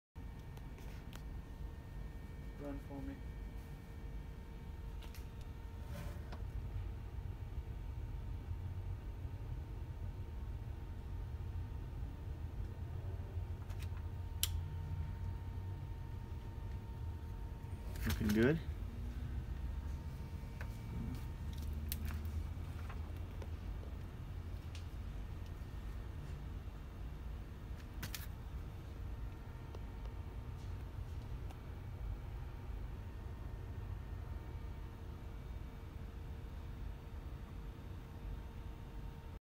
Redwing Tuff Toe
Watching how Redwing applies Tuff Toe to some used boots.